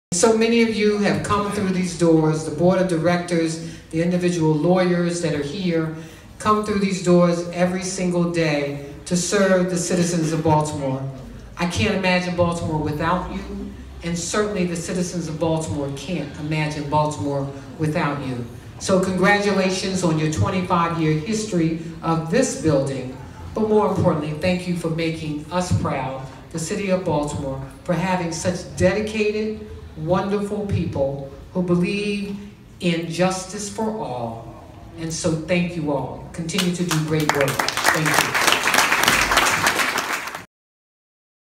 Maryland Legal Aid Re-Dedicates Baltimore HQ
"I can't imagine Baltimore without you," the city's mayor, Catherine Pugh, told the crowd of legal luminaries who packed the lobby of Maryland Legal Aid's Baltimore headquarters on October 5, 2017, to celebrate the building's 25th anniversary.